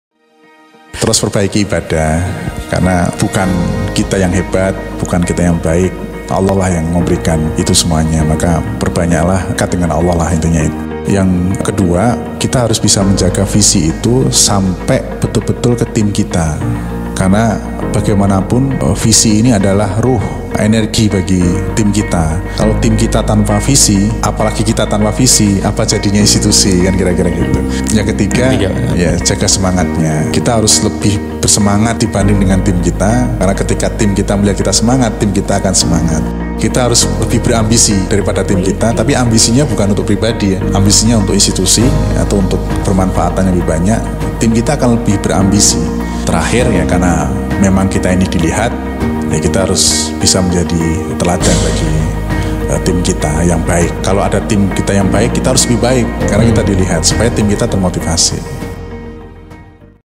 SUKSES DENGAN BAIK ALA CEO RUMAH ZAKAT
Sahabat, di balik kesuksesan Rumah Zakat dalam membahagiakan mereka yang membutuhkan tentu saja ada peran besar dari pemimpinnya.

Nah, kali ini pak Nur Effendi selaku CEO Rumah Zakat memberikan tips untuk kita untuk bertumbuh dan berkembang dalam meraih kesuksesan.

Penasaran seperti apa? Yuk, cek video berikut ini.